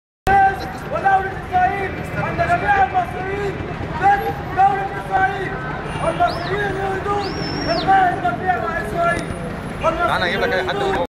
طلاب جامعة القاهرة يتظاهرون أمام سفارة إسرائيل
Cairo University students demonstrating in front of the Israeli embassy in Cairo, demanding cutting political and economic ties with the Zionist Apartheid state.